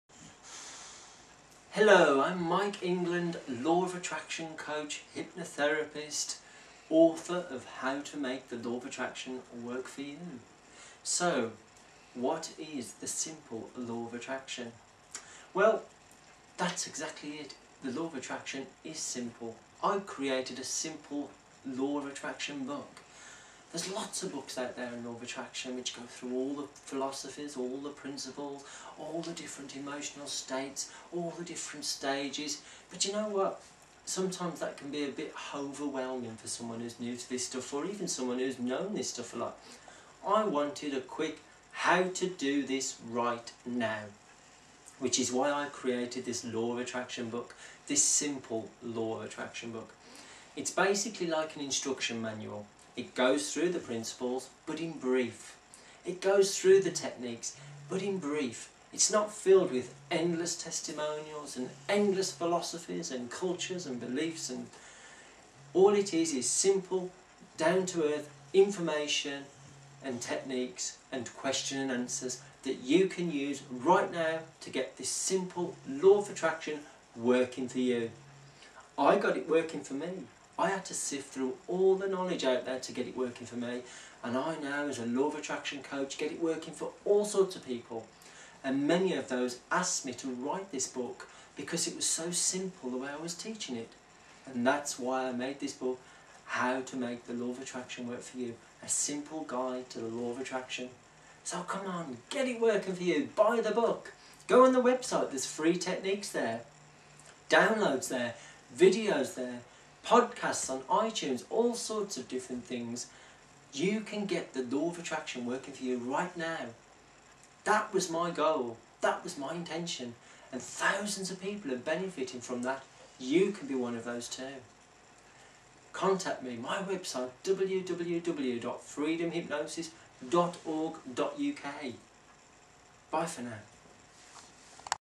The Simple Law of Attraction
The law of attraction is really very simple, don't over complicate things, remember to focus and especially get emotionally involved with the things you want in your life.